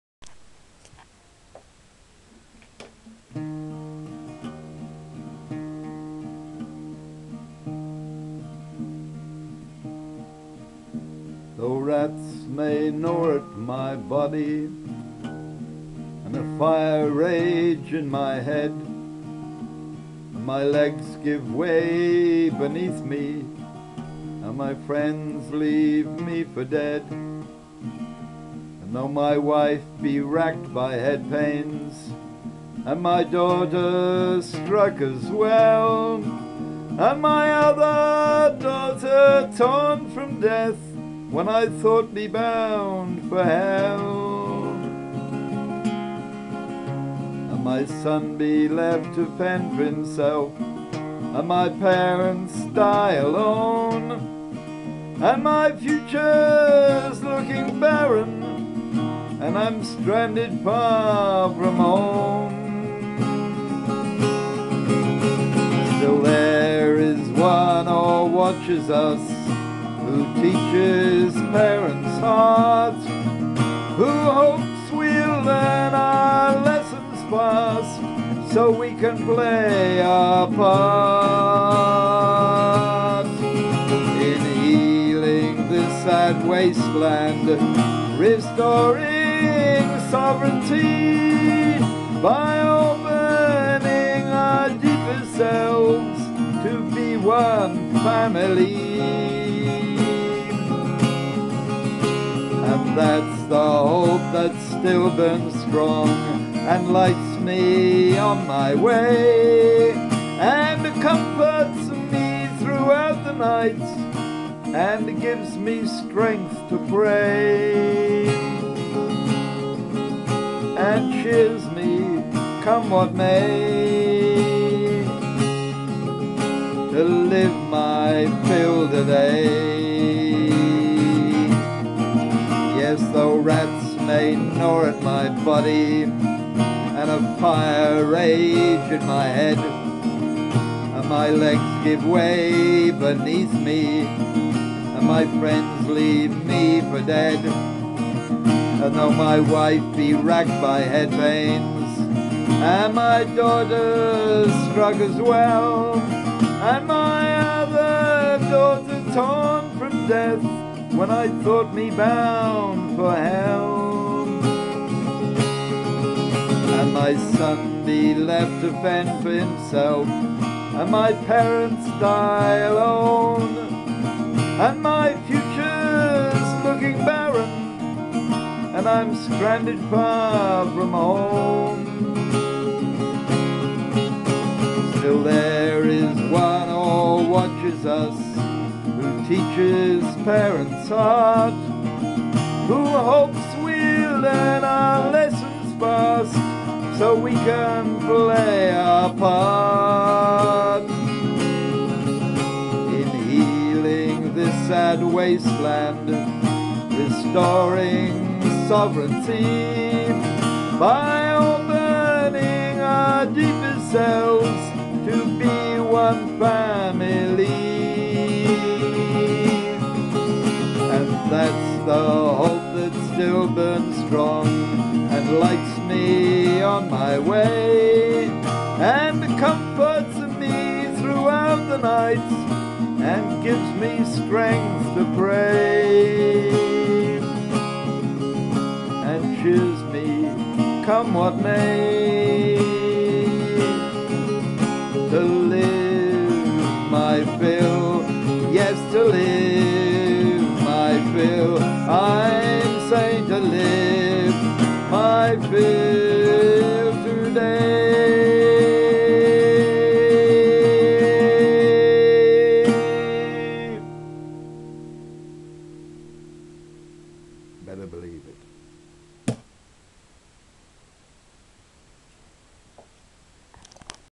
To live my fill today - though rats may gnaw at my belly
To live my fill today - though rats may gnaw at my belly: Christopher Vipond Davies, June 19, 1994, London. The first line is about a dream my eldest daughter had about me...